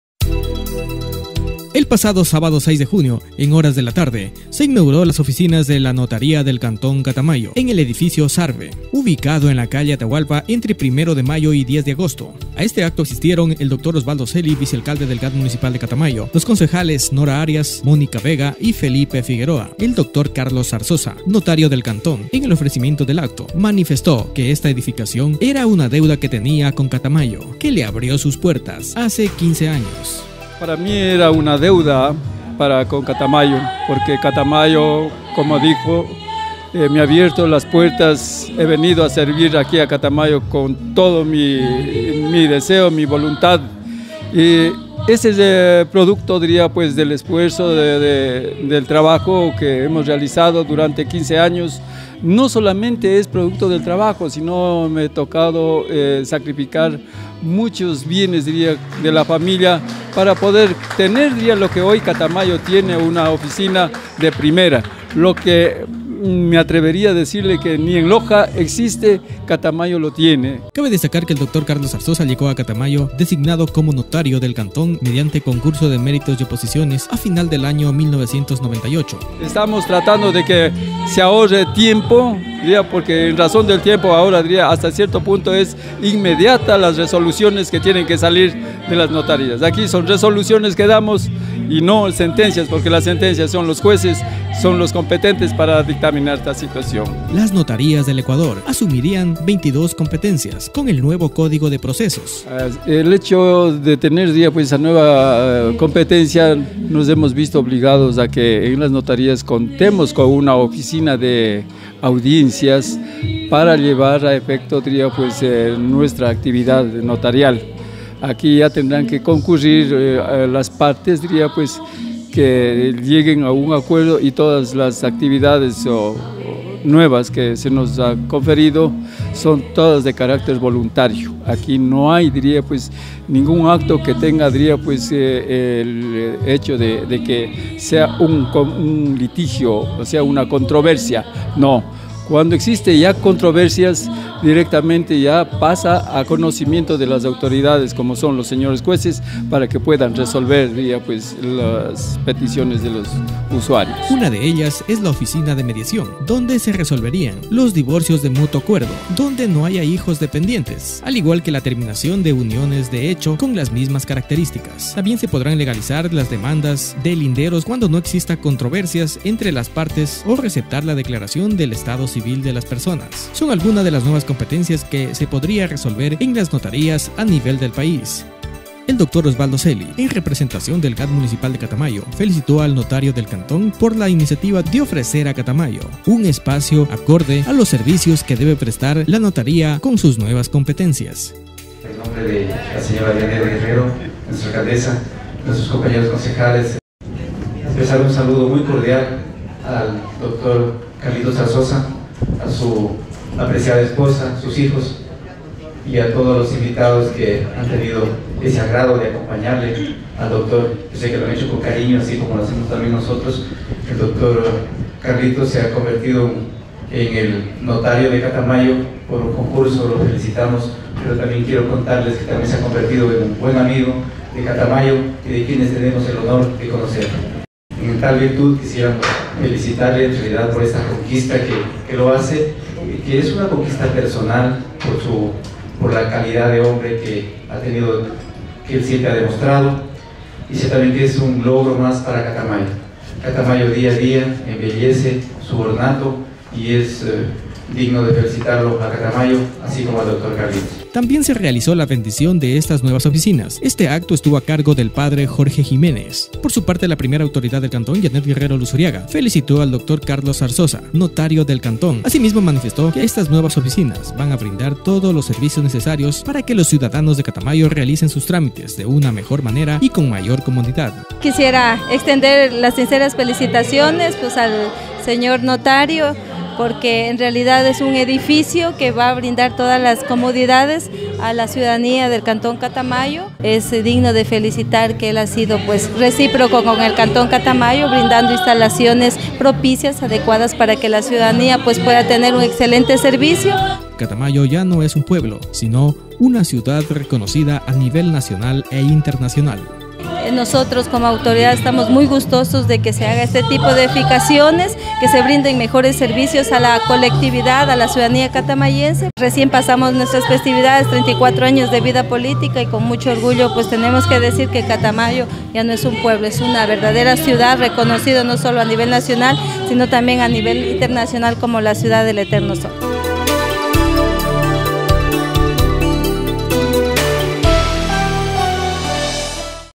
Catamayo.-│Notaria de cantón inaugura nuevas oficinas
El pasado sábado 06 de junio de 2015, en horas de la tarde se inauguró las oficinas de la Notaria del Cantón en el Edificio SARBE, ubicado en la calle Atahualpa, entre Primero de mayo y 10 de agosto.
A este acto asistieron el Dr. Oswaldo Celi, Vicealcalde GADM Catamayo, los concejales Nora Arias, Mónica Vega y Felipe Figueroa.